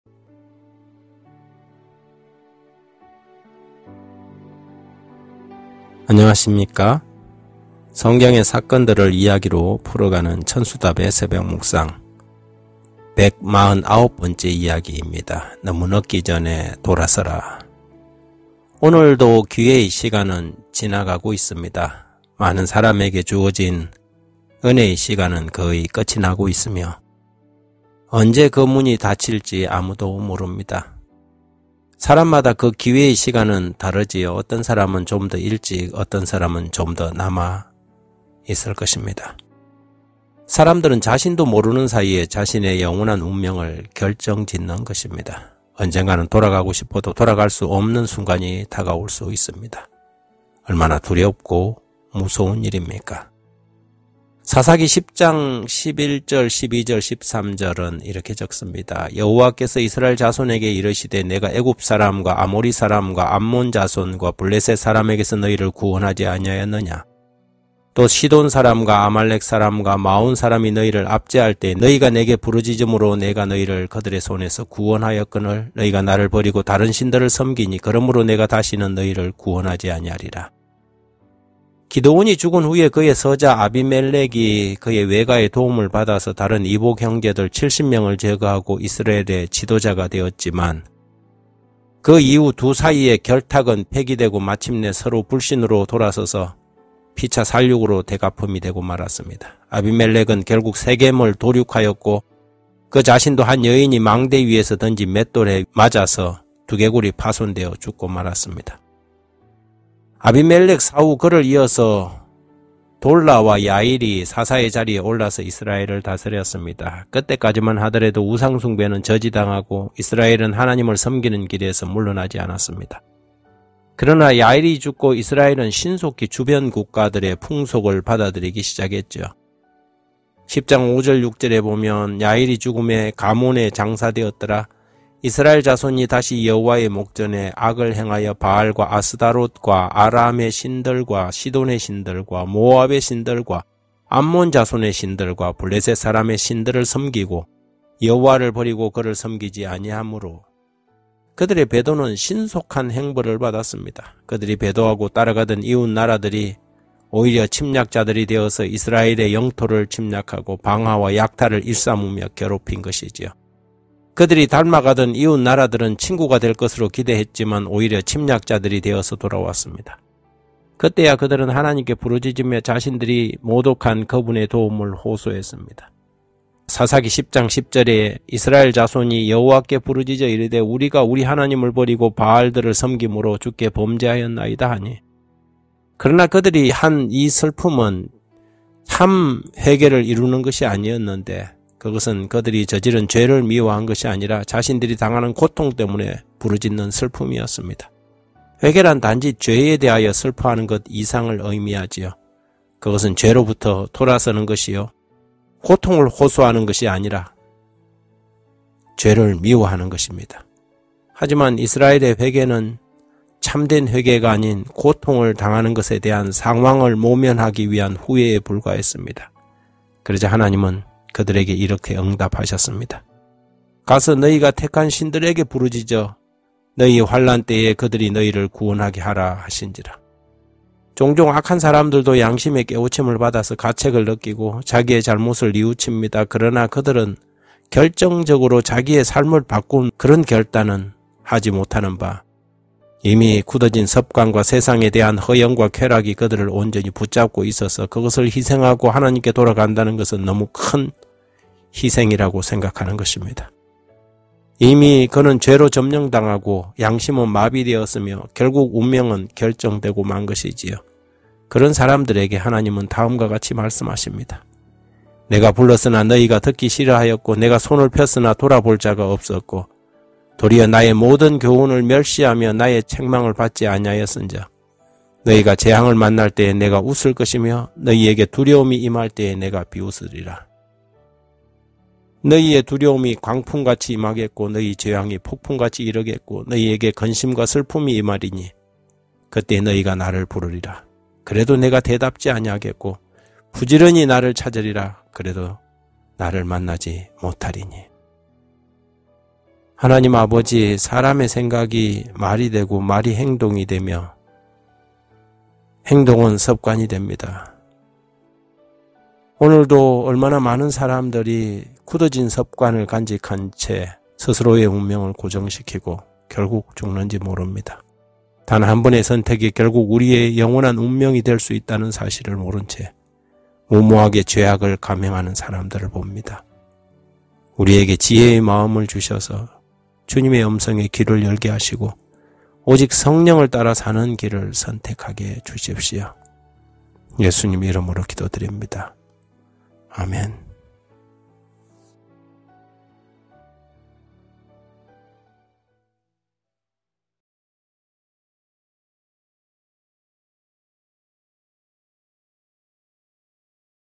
백마흔아홉 번째 이야기, 너무 늦기 전에 돌아서라(천수답의 새벽묵상)
더 깊은 성경연구로 당신을 초대합니다.
쉽고 재미있는 성경연구

백마흔아홉 번째 이야기, 너무 늦기 전에 돌아서라

오늘도 기회의 시간은 지나가고 있다. 많은 사람에게 주어진 은혜의 시간은 거의 끝나가고 있으며 언제 그 문이 닫힐지 아무도 모른다. 사람마다 그 기회의 시간은 다르다. 어떤 사람은 좀 더 일찍, 어떤 사람은 좀 더 남아있다. 사람들은 자신도 모르는 사이에 자신의 영원한 운명을 결정짓는 것이다. 언젠가는 돌아가고 싶어도 돌아갈 수 없는 순간이 다가올지 모른다. 얼마나 두렵고 무서운 일인가?

(삿 10:11) 여호와께서 이스라엘 자손에게 이르시되 내가 애굽 사람과 아모리 사람과 암몬 자손과 블레셋 사람에게서 너희를 구원하지 아니하였느냐
(삿 10:12) 또 시돈 사람과 아말렉 사람과 마온 사람이 너희를 압제할 때에 너희가 내게 부르짖으므로 내가 너희를 그들의 손에서 구원하였거늘
(삿 10:13) 너희가 나를 버리고 다른 신들을 섬기니 그러므로 내가 다시는 너희를 구원하지 아니하리라

기드온이 죽은 후에 그의 서자 아비멜렉이 그의 외가의 도움을 받아서 다른 이복형제들 칠십 명을 제거하고 이스라엘의 지도자가 되었지만, 그 이후 두 사이의 결탁은 폐기되고 마침내 서로 불신으로 돌아서고 피차 살육으로 되갚음이 되고 말았다. 아비멜렉은 결국 세겜을 도륙하고 그 자신도 한 여인이 망대 위에서 던진 맷돌 위짝에 맞아서 두개골이 파손되어 죽고 말았다.

아비멜렉 사후 그를 이어서 돌라와 야일이 사사의 자리에 올라서 이스라엘을 다스렸다. 그때까지만 하더라도 우상숭배는 저지당하고 이스라엘은 하나님을 섬기는 길에서 물러나지 않았다. 그러나 야일이 죽자 이스라엘은 신속히 주변 국가들의 풍속을 받아들이기 시작했다.

(삿 10:5) 야일이 죽으매 가몬에 장사되었더라
(삿 10:6) 이스라엘 자손이 다시 여호와의 목전에 악을 행하여 바알들과 아스다롯과 아람의 신들과 시돈의 신들과 모압의 신들과 암몬 자손의 신들과 블레셋 사람들의 신들을 섬기고 여호와를 버리고 그를 섬기지 아니하므로

그들의 배도는 신속한 형벌을 받았다. 그들이 배도하고 따라가던 이웃 나라들이 오히려 침략자들이 되어서 이스라엘의 영토를 침략하고 방화와 약탈을 일삼으며 괴롭힌 것이다. 그들이 닮아가던 이웃 나라들은 친구가 될 것으로 기대했지만 오히려 침략자들이 되어서 돌아왔다. 그때야 그들은 하나님께 부르짖으며 자신들이 모독한 그분께 도움을 호소하였다. (삿 10:10) 이스라엘 자손이 여호와께 부르짖어 이르되 우리가 우리 하나님을 버리고 바알들을 섬김으로 주께 범죄하였나이다 하니

그러나 그들의 슬픔은 참 회개를 이루는 것이 아니었는데 그것은 그들이 저지른 죄를 미워한 것이 아니라 자신들이 당하는 고통 때문에 부르짖는 슬픔이었다. 회개란 단지 죄에 대하여 슬퍼하는 것 이상을 의미한다. 그것은 죄로부터 돌아서는 것이요 고통을 회피하는 것이 아닌 죄를 미워하는 것이다. 하지만 이스라엘의 회개는 참된 회개가 아닌 고통을 당하는 것에 대한 상황을 모면하기 위한 후회에 불과했다. 그러자 하나님은 그들에게 이렇게 응답하셨다.

(삿 10:14) 가서 너희가 택한 신들에게 부르짖어 너희의 환난 때에 그들이 너희를 구원하게 하라 하신지라

종종 악한 사람들도 양심의 깨우침을 받아서 가책을 느끼고 자기의 잘못을 뉘우친다. 그러나 그들은 결정적으로 자기의 삶을 바꿀 만큼 결단하지 못하는바 이미 굳어진 습관과 세상에 대한 허영과 쾌락이 그를 온전히 붙잡고 있어서 모든 것을 희생하고 하나님께 돌아간다는 것은 그에게 너무 큰 요구라고 생각하는 것이다. 이미 그는 죄로 점령당하고 양심은 마비되었으며 결국 운명은 결정되고 만 것이다. 그런 사람들에게 하나님은 다음과 같이 말씀하신다.

(잠 1:24) 내가 불렀으나 너희가 듣기 싫어하였고 내가 손을 폈으나 돌아보는 자가 없었고
(잠 1:25) 도리어 나의 모든 교훈을 멸시하며 나의 책망을 받지 아니하였은즉
(잠 1:26) 너희가 재앙을 만날 때에 내가 웃을 것이며 너희에게 두려움이 임할 때에 내가 비웃으리라
(잠 1:27) 너희의 두려움이 광풍같이 임하겠고 너희의 재앙이 폭풍같이 이르겠고 너희에게 근심과 슬픔이 임하리니
(잠 1:28) 그 때에 너희가 나를 부르리라 그래도 내가 대답하지 아니하겠고 부지런히 나를 찾으리라 그래도 나를 만나지 못하리니

사람의 생각이 말이 되고 말이 행동이 되며 행동이 습관이 됩니다.
오늘도 얼마나 많은 사람이 굳어진 습관을 간직한 채로 스스로의 운명을 고정시키고
결국 죽는지 모릅니다. 단 한 번의 선택이 결국 우리의 영원한 운명이 될 수 있다는 사실을
모른 채 무모하게 죄악을 감행하는 사람들을 봅니다.
우리에게 지혜의 마음을 주셔서
주님의 음성에 귀를 열게 하시고
오직 성령을 따라 사는 길을 선택하게 하소서.
예수님의 이름으로 기도드립니다.
아멘.